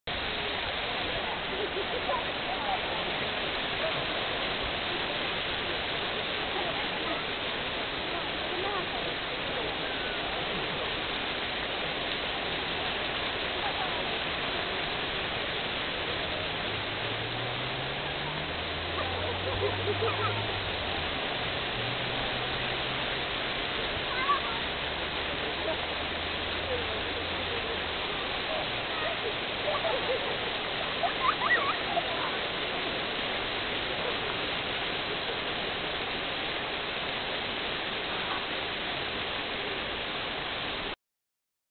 Rory in summer
Rory plays in the fountain at Balatonfured